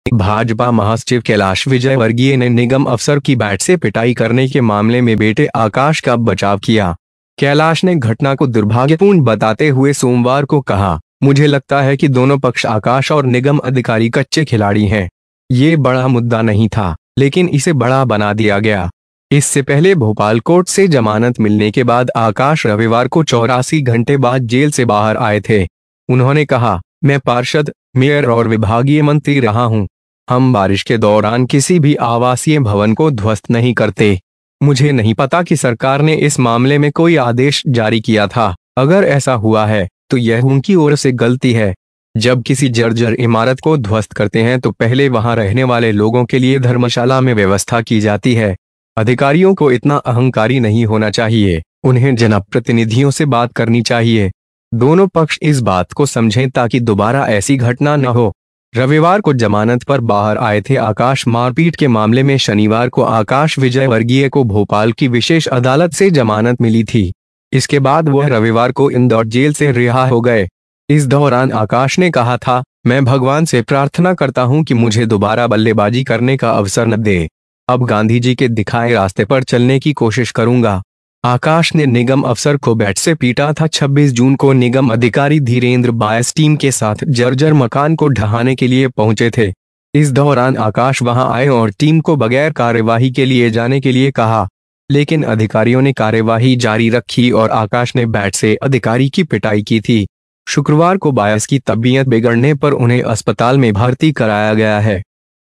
विजयवर्गीय ने निगम अफसरों को बताया कच्चा खिलाड़ी
BJP General Secretary Kailash Vijayvargiya defended son Akash in the case of beating with the Corporation Officer's Bat. Kailash said on Monday, saying the incident was "unfortunate", "I think both parties Sky and Corporation officials are raw players. This was not a big issue, but it was made bigger. Earlier, after getting bail from Bhopal court, Akash came out of jail after 84 hours on Sunday. He said, I have been a councilor, mayor and departmental minister. We do not demolish any residential building during the rain. I do not know if the government had issued an order in this matter, if it has happened, then it is a mistake on their part. When you destroy a shabby building, arrangements are made in 'Dharamsala' for those who live there first. The authorities should not be so arrogant, they should talk to the people. Both sides understand this fact that there should not be such an incident again. On Sunday, in the case of sky war, Akash Vijayvargiya got bail from Bhopal special court on Saturday. After this he was released from Indore Jail on Sunday. During this, Akash said, 'I pray to God that I do not give opportunity to bat again. Now, I will try to follow the path shown by Gandhiji. The sky was beaten by Bait with the corporation officer. On 26th June, corporator Dhirendra Baius reached the team to demolish a shabby house. During this the sky came and told the team to go without action. But the officials continued the operation and Akash beat the officer from Bat. On Friday, he was admitted to the hospital after biases of his health deteriorated.